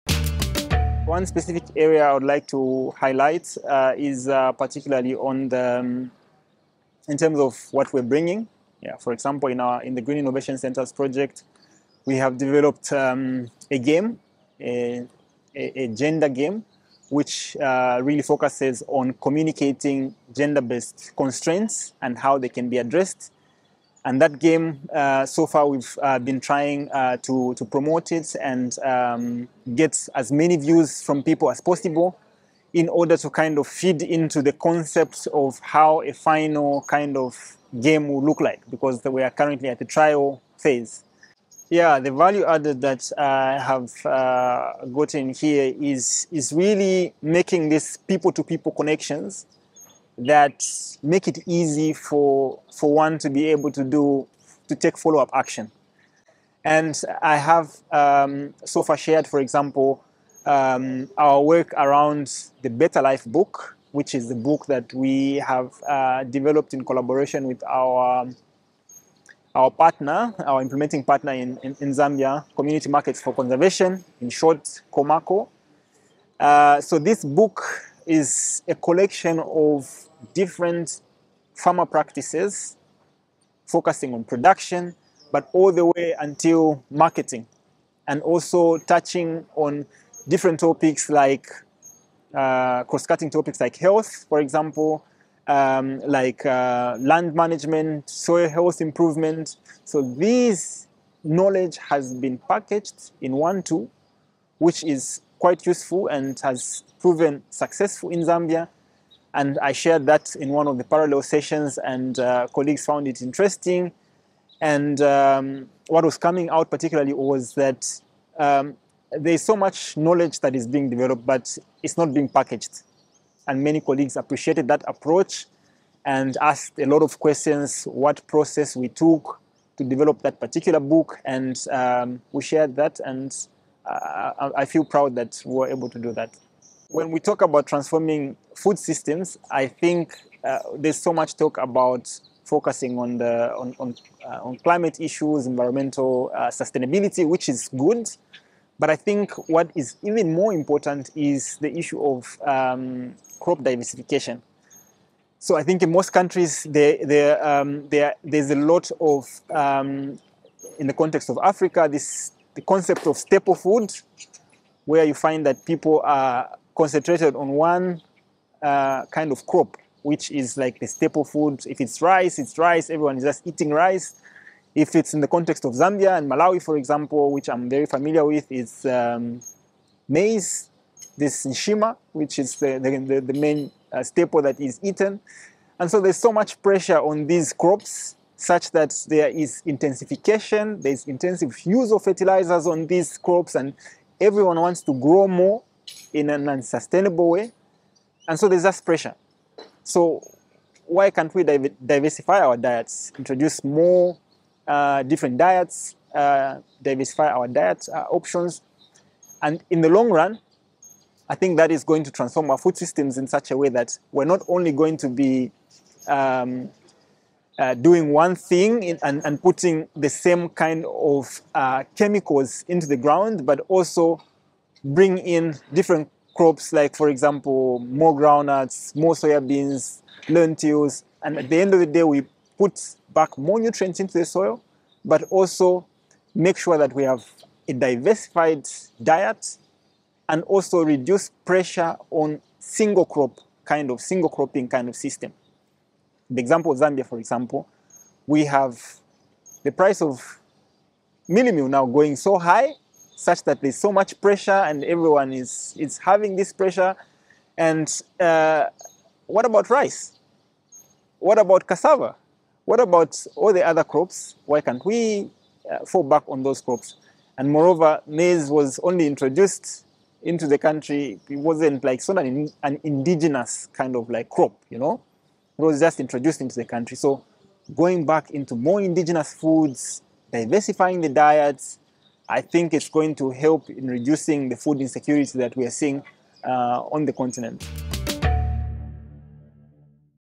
Crop Diversification to Promote More Sustainable Agri-Food Systems
✅ Brief statement by Mwansa Mungela at the 2023 SNRD Africa conference "Transform & Innovate: Future-proof Food Systems", which was held in Somone/Senegal, 8-13 October.

Mwansa is Advisor Gender & Sustainability Green Innovation Centre in Zambia and a co-speaker for the working group Gender Transformative Approaches.

SNRD Africa is short for Sector Network Rural Development Africa.